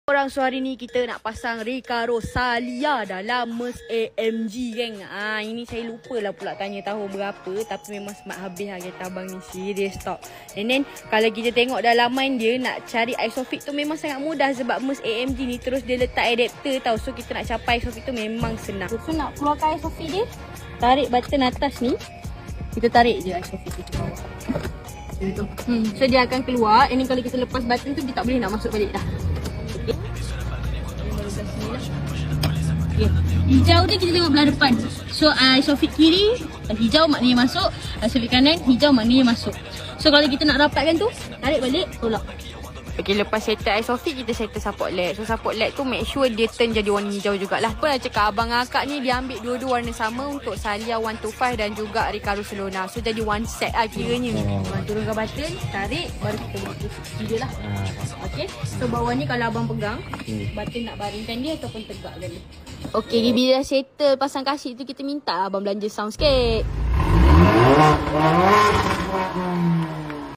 Install Recaro Salia in Mercedes AMG 45 (Live Demo)
Installa Recaro Salia into Mercedes AMG 45